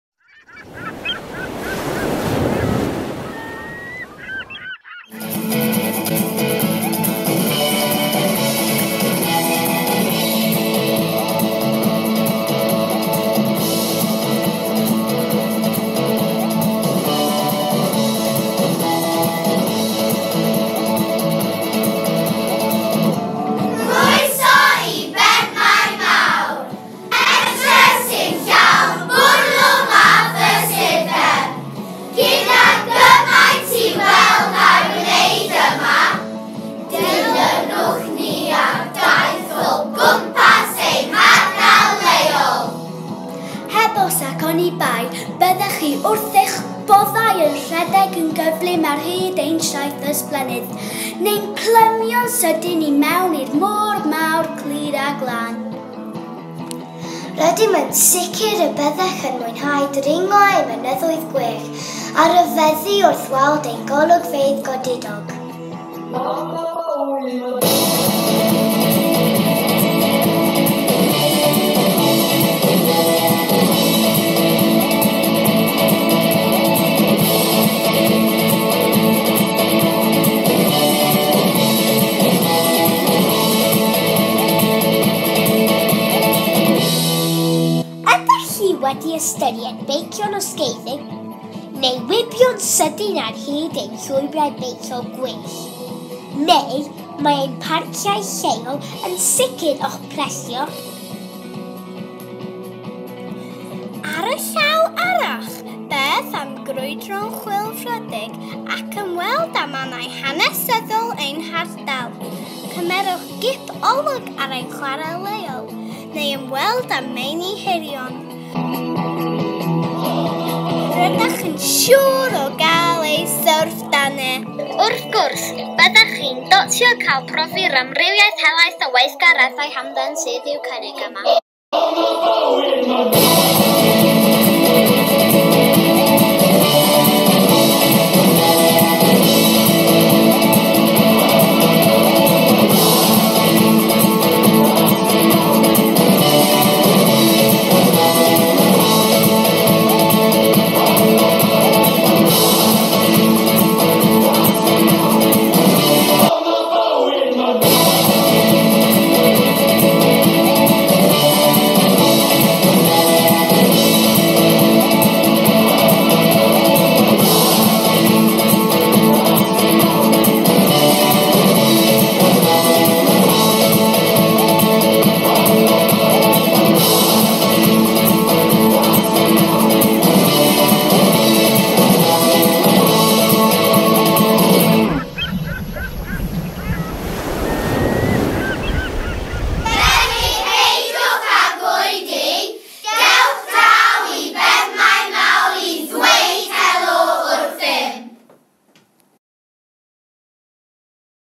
Addysg : Cymhwysedd Ddigidol : Dehongli ArdaL Leol - neu 'Rough Guide'
Project Ein Bro Eisteddfod Sir Conwy 2019.
Disgyblion Ysgol Pencae Penmaenmawr yn dehongli eu bro, ac yn croesawy ymwelwyt i'r ardal yn ystod Eisteddfod Conwy 2019.